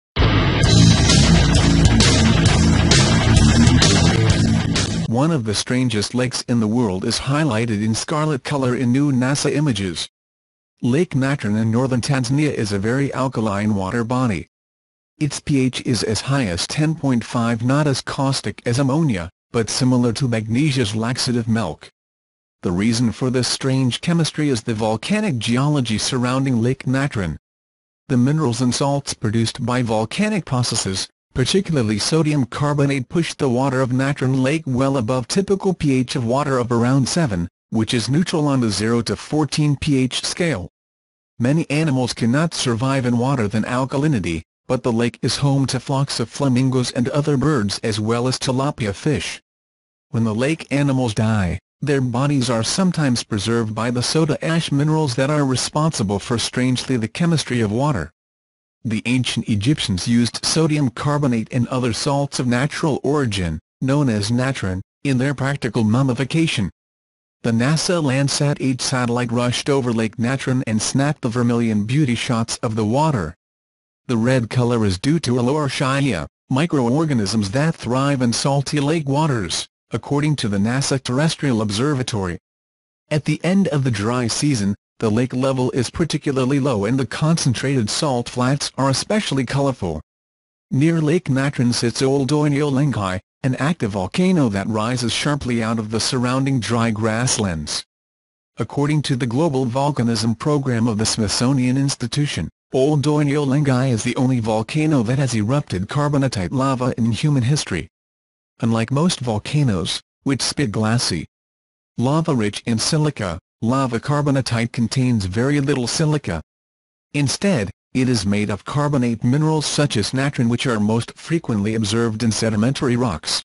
Lake Natron: One of the strangest lakes in the world stands out in scarlet color.
One of the strangest lakes in the world is highlighted in scarlet color in new NASA images. Lake Natron in northern Tanzania is a very alkaline water body. Its pH is as high as 10.5 not as caustic as ammonia, but similar to Magnesia's laxative milk. The reason for this strange chemistry is the volcanic geology surrounding Lake Natron. The minerals and salts produced by volcanic processes, particularly sodium carbonate push the water of Natron Lake well above typical pH of water of around 7, which is neutral on the 0 to 14 pH scale.

Many animals can not survive in water than alkalinity, but the lake is home to flocks of flamingos and other birds as well as tilapia fish. When the lake animals die, their bodies are sometimes preserved by the soda ash minerals that are responsible for strangely the chemistry of water. The ancient Egyptians used sodium carbonate and other salts of natural origin, known as natron, in their practical mummification.

The NASA Landsat 8 satellite rushed over Lake Natron and snapped the vermilion beauty shots of the water. The red color is due to haloarchaea, microorganisms that thrive in salty lake waters, according to the NASA Terrestrial Observatory. At the end of the dry season, the lake level is particularly low and the concentrated salt flats are especially colorful.

Near Lake Natron sits Ol Doinyo Lengai, an active volcano that rises sharply out of the surrounding dry grasslands. According to the Global Volcanism Program of the Smithsonian Institution, Ol Doinyo Lengai is the only volcano that has erupted carbonatite lava in human history. Unlike most volcanoes, which spit glassy, ​​lava rich in silica, lava carbonatite contains very little silica. Instead, it is made of carbonate minerals such as natron which are most frequently observed in sedimentary rocks.